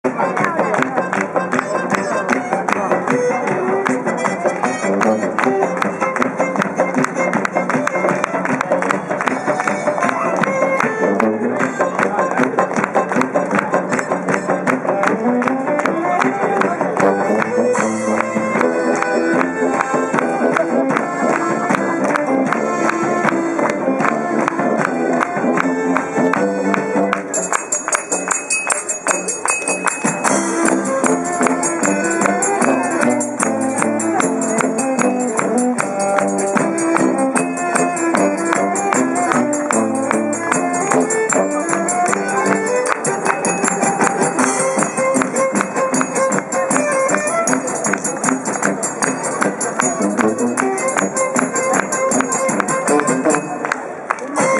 大昭和祭り・ちんどんバンド★ざくろ（ステージ１）
2014年4月29日、白河市、大昭和祭り、ちんどんバンド★ざくろさんのステージ（その１）